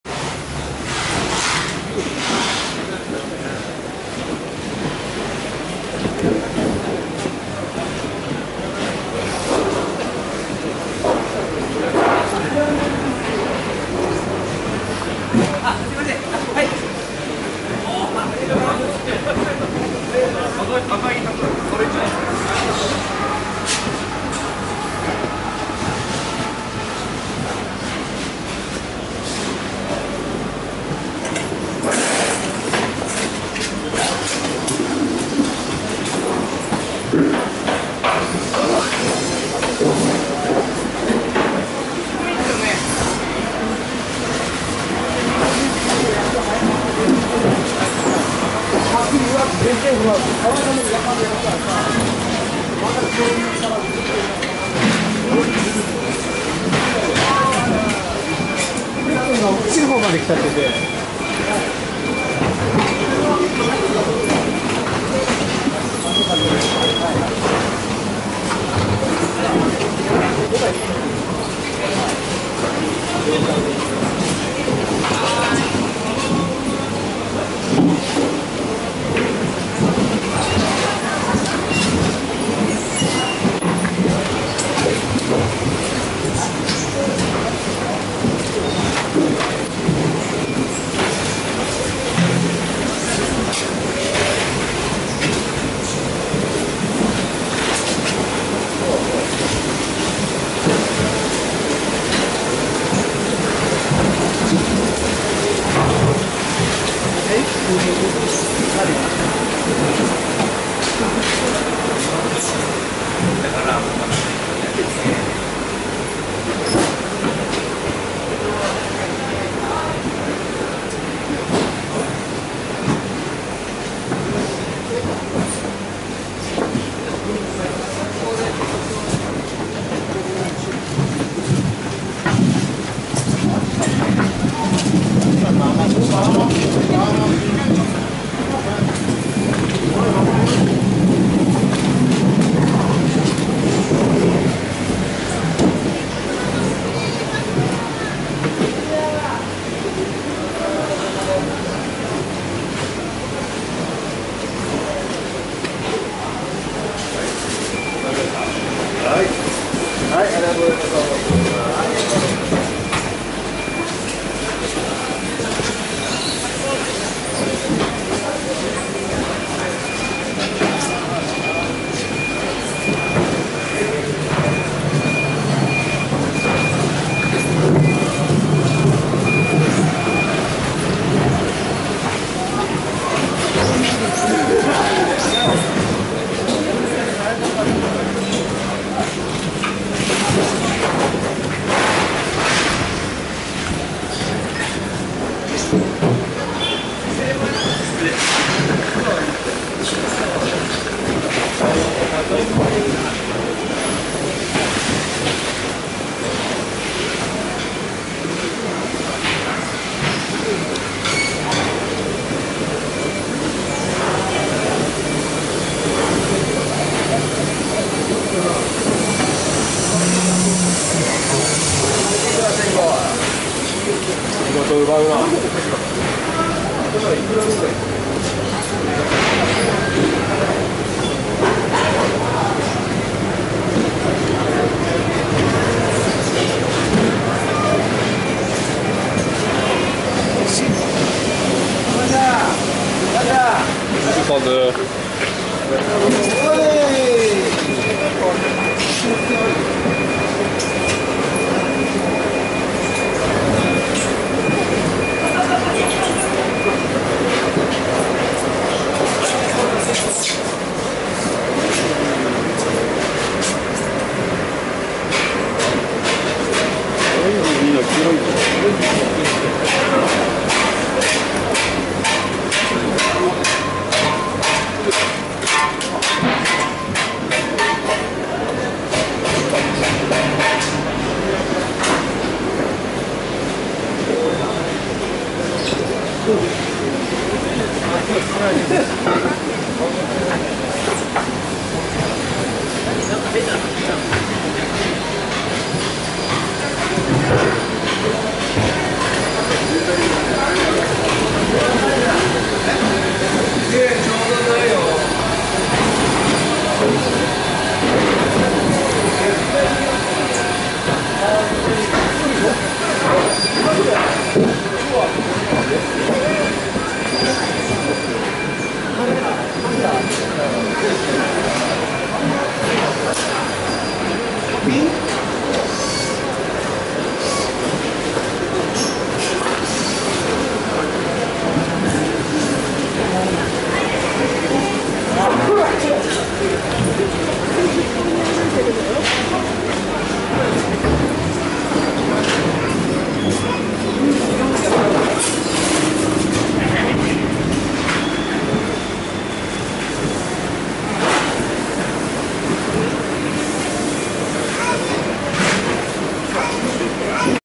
Tsukiji Fish Market
The video attempts to show the ebourmous size of the Tsukiji fish market.